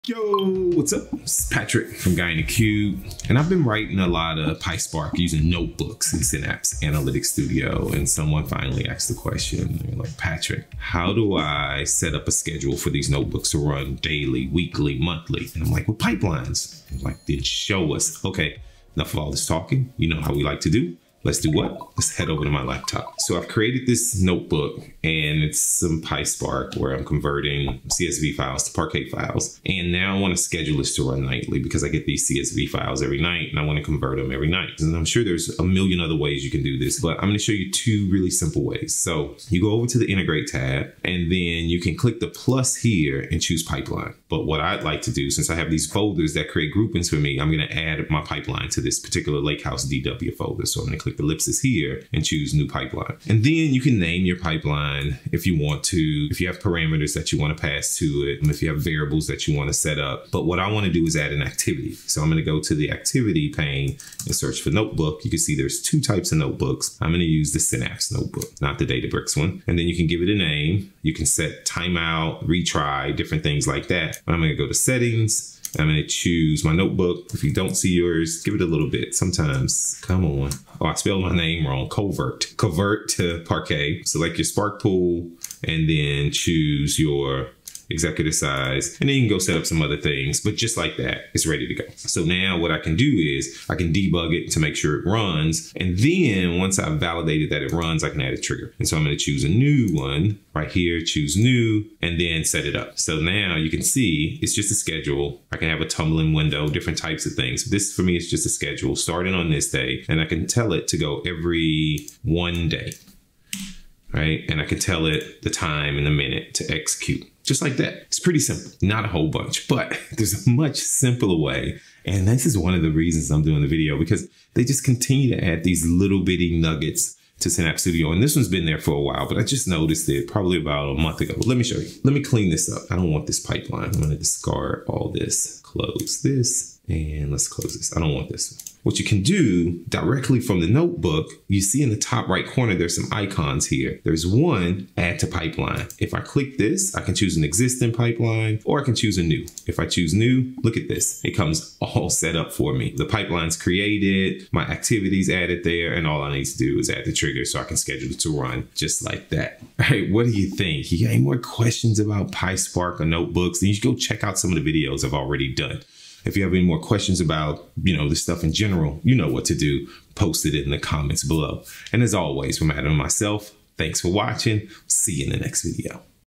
How to SCHEDULE your notebooks to run with Azure Synapse
Taking advantage of Notebooks in Azure Synapse Analytics? How do you automate that great work you did? Patrick shows you some options to schedule the run of your notebooks.